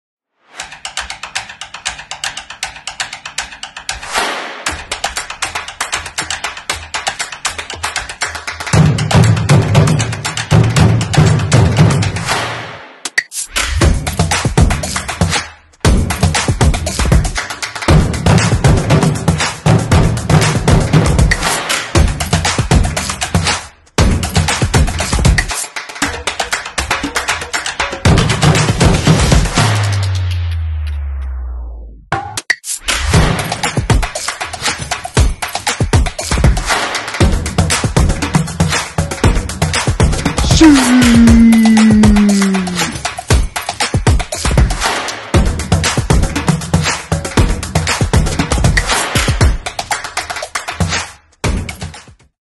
FUSSBALL DAHOAM - Star-Challenge - Cristiano Ronaldo
Felix fordert heute zur Star-Challenge. Wer wird der neue Ronaldo und klaut Felix seine Krone?